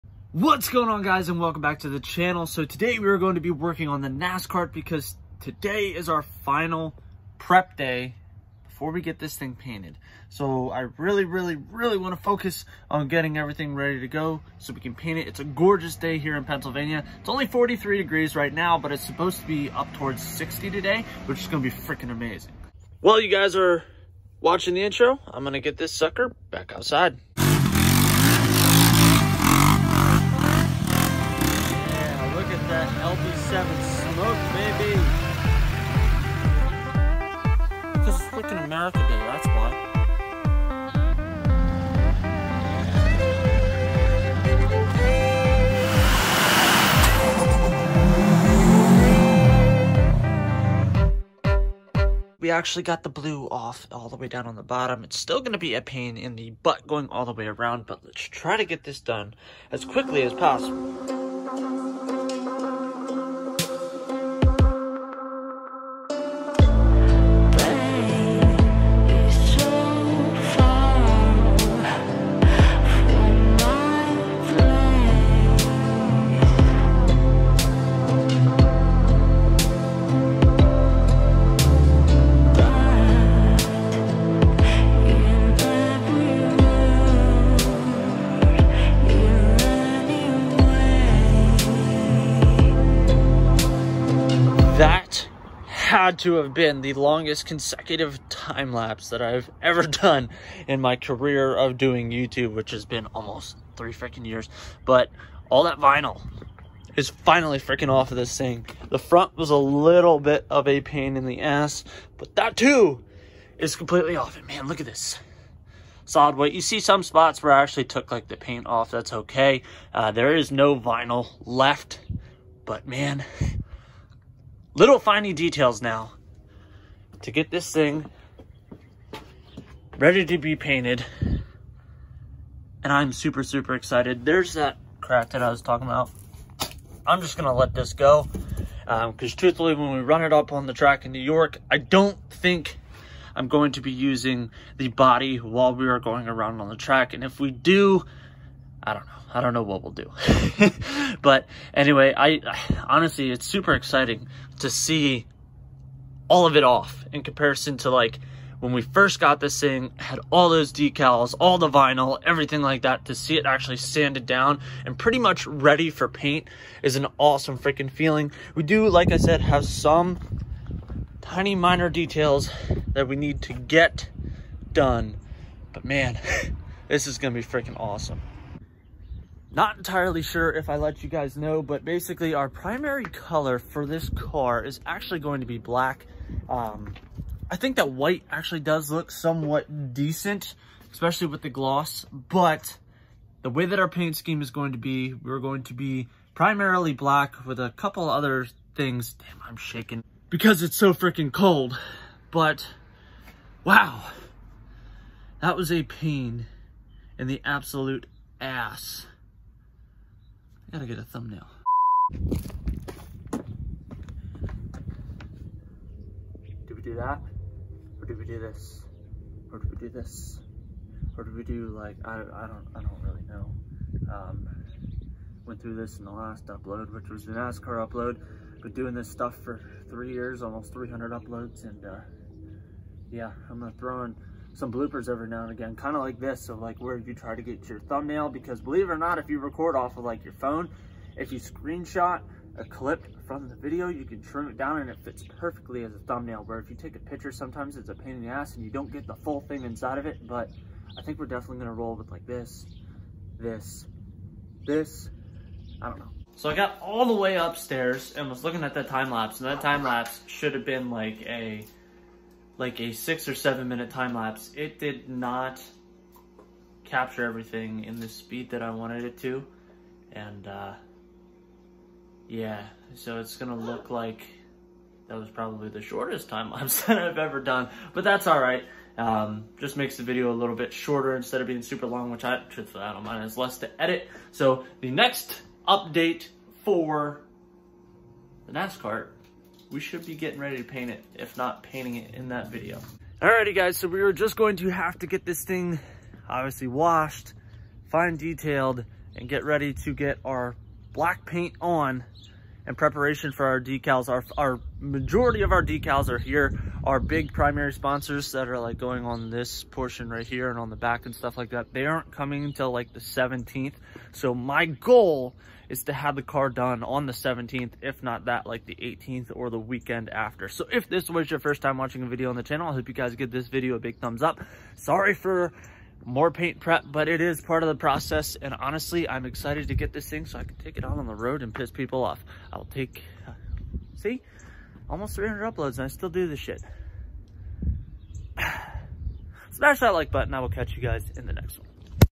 The NASKART Is Ready For Paint!! ( nascar go kart build)
In today’s video we finally get the NASKART the nascar go kart finished up and ready for paint! My final decals will be showing up by the 17th so I’m hoping you have the go kart completely finished during that week!